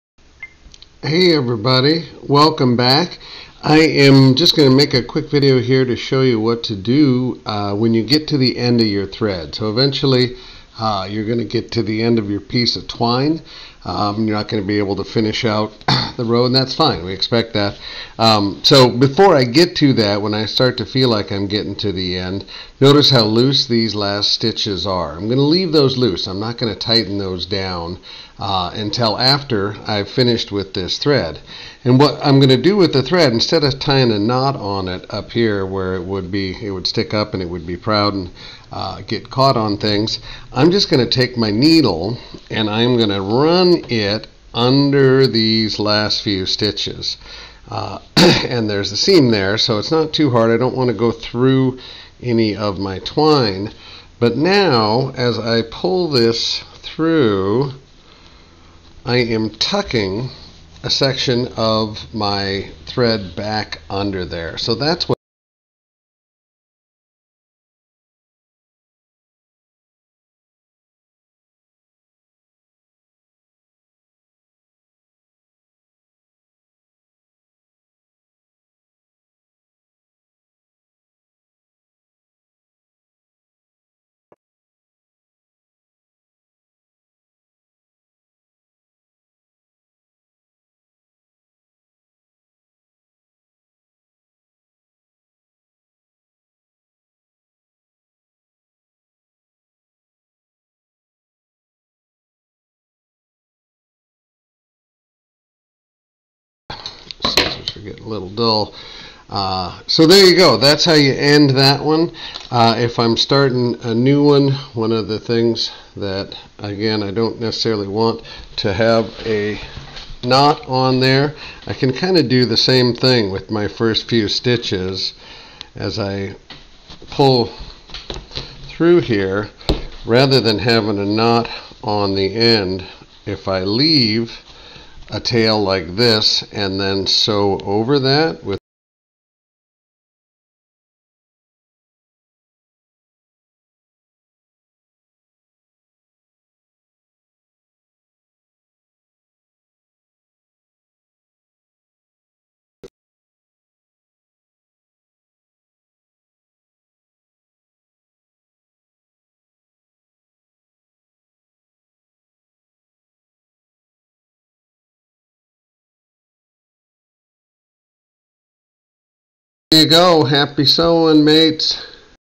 Ditty Bag Project - How to Start New Thread
This video demonstrates what to do when you run out of thread while sewing the flat stitch on the long seam of your ditty bag. It covers finishing the stitch, as well as starting with a new thread.